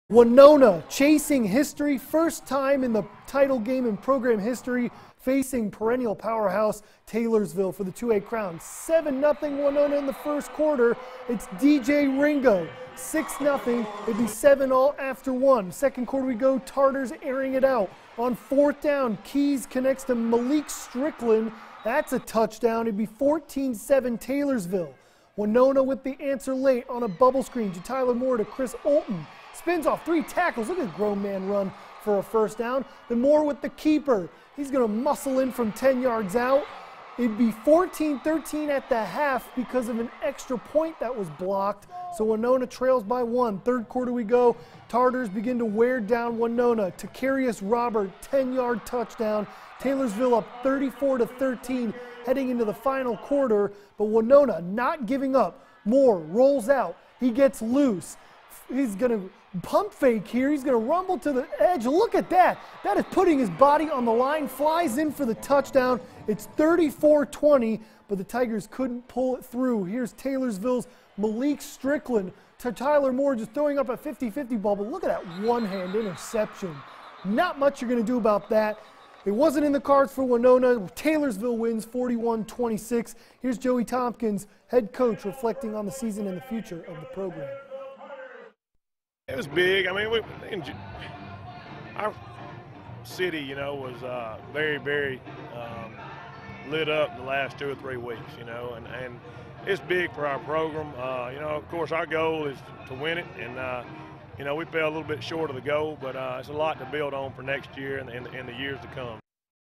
Taylorsville Too Much For Winona
Winona falls short in the 2A state championship, falling 41-26.